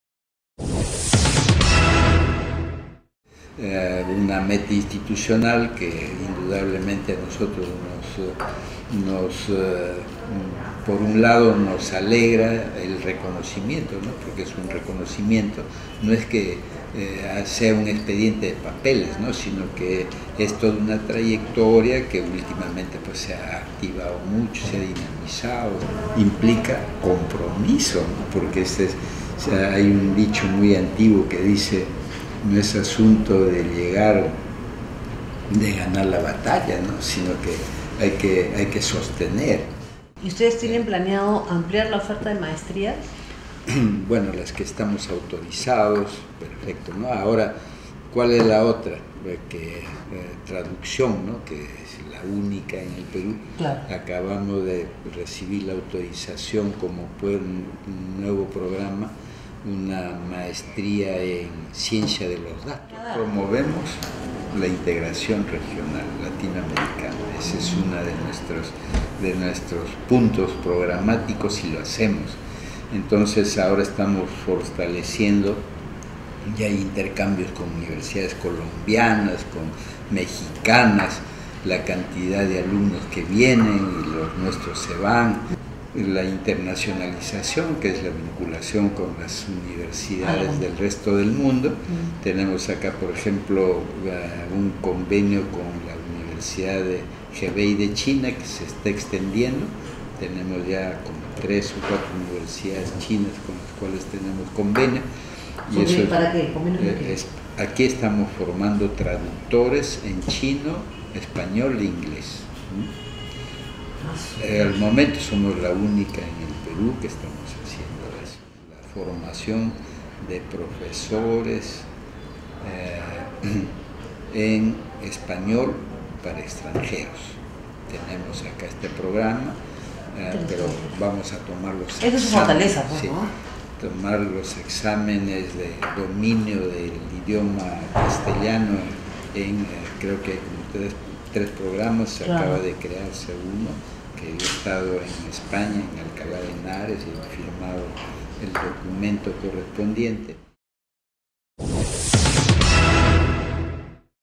U. Ricardo Palma logra licenciamiento de Sunedu
Universidad Ricardo Palma firmará convenios de estudios con universidades extranjeras.
URP obtuvo licenciamiento de Sunedu  y fortalecerá su oferta educativa con países de la región y China.
La Universidad Ricardo Palma firmará convenios con universidades de América latina y China con miras a desarrollar programas de intercambio de estudios, maestrías y la formación de traductores informó el rector, Iván Rodríguez Chávez.